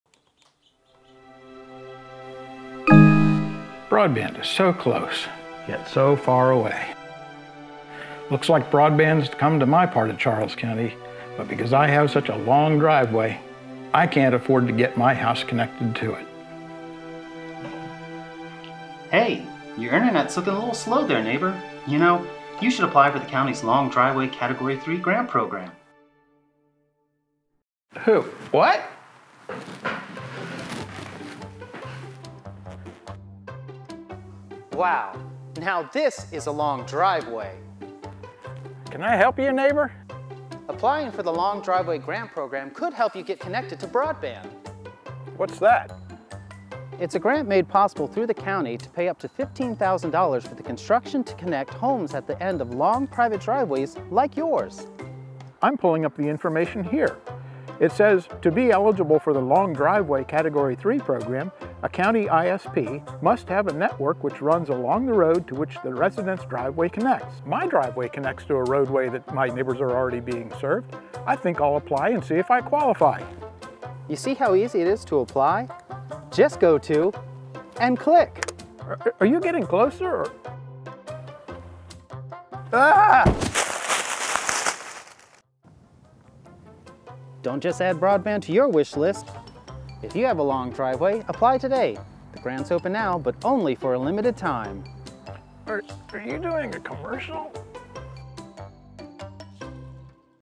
Charles County Long Driveway(CAT3) Grant Program
It’s a grant made available through the County that could pay up to 15 thousand dollars for the construction to connect homes at the end of long private driveways. Apply Today! Restrictions may apply!